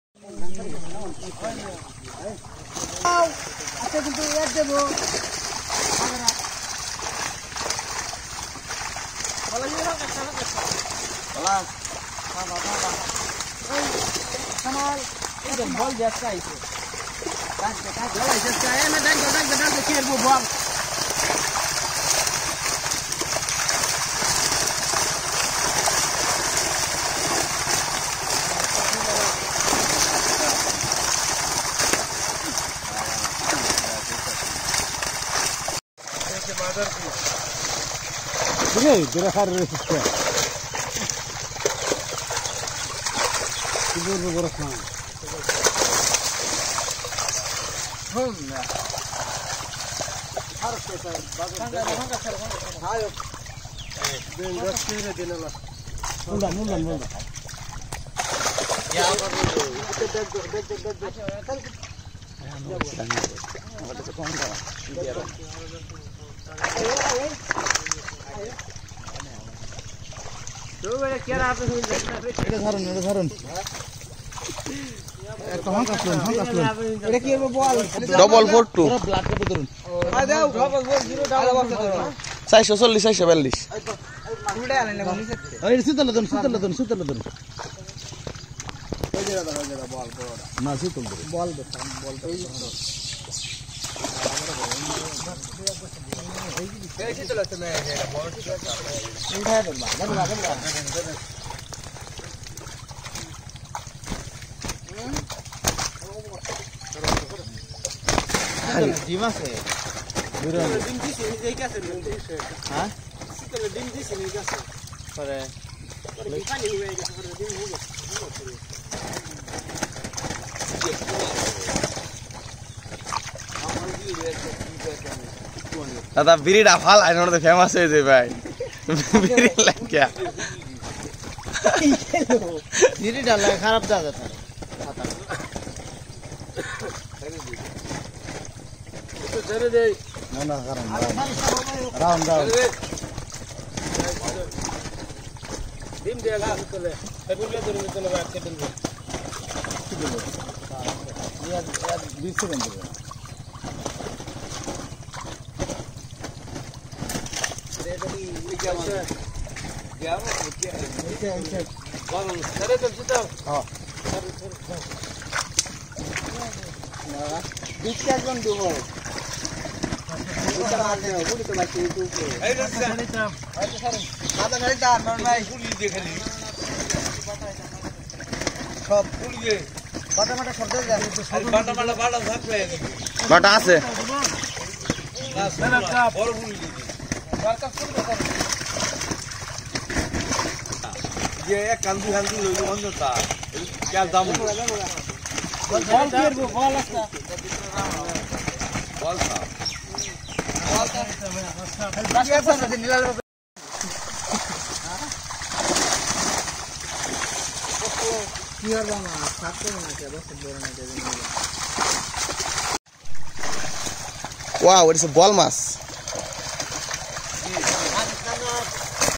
Boal and chital fish catching
Boal and chital fish catching, It is a 6-month fish farm project.